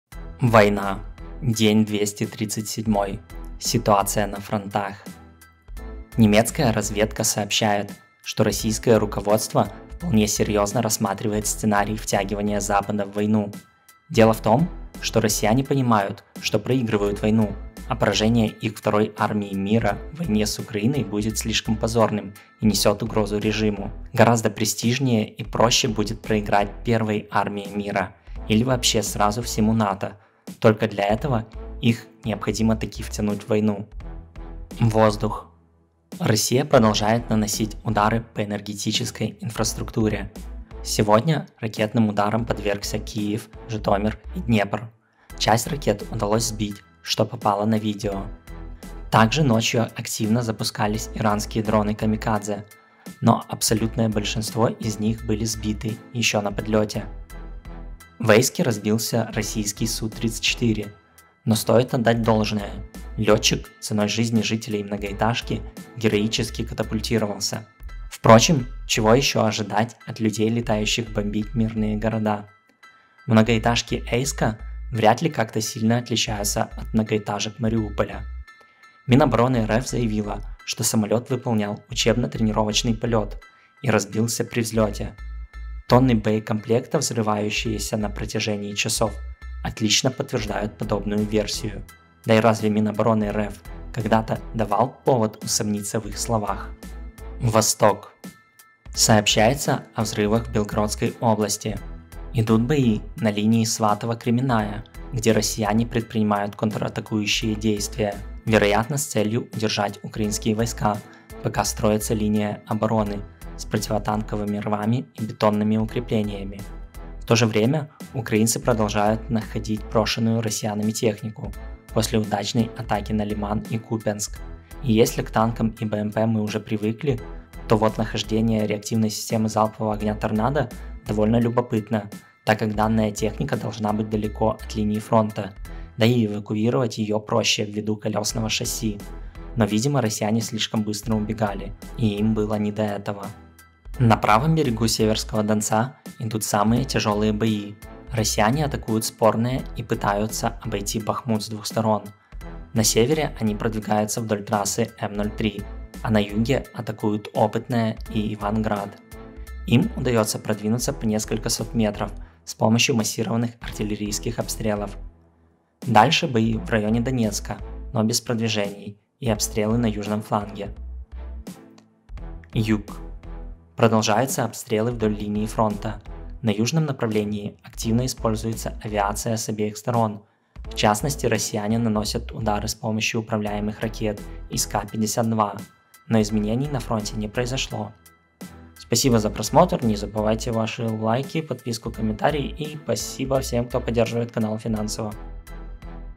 Война. 237-й день. Ситуация на фронтах. Падение Су-34 в Ейске.
Война России с Украиной. 237-й день вторжения. Ситуация на фронтах на 18 октября 2022 года. День 237. Падение Су-34 в Ейске.

Карта Приват Банк грн: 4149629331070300